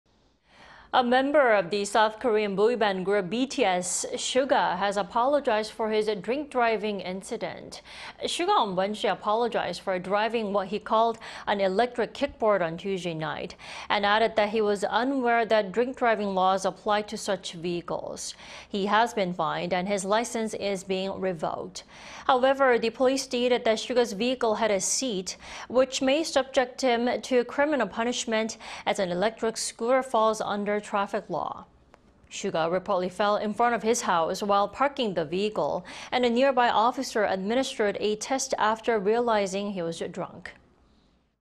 BTS Suga apologizes for drink driving incident
BTS 슈가 측, '전동 스쿠터 음주운전' 재차 사과 "사안 축소 의도 없어"

A member of the South Korean boyband group BTS, Suga has apologized over his drink driving incident.
Suga on Wednesday apologized for driving what he called an "electric kickboard" on Tuesday night and added that he was unaware that drink driving laws applied to such vehicles.
He has been fined and his license is being revoked.
However, the police stated that Suga's vehicle had a seat which may subject him to criminal punishment as an electric scooter falls under traffic law.
Suga reportedly fell in front of his house while parking the vehicle, and a nearby officer administered a test after realizing he was drunk.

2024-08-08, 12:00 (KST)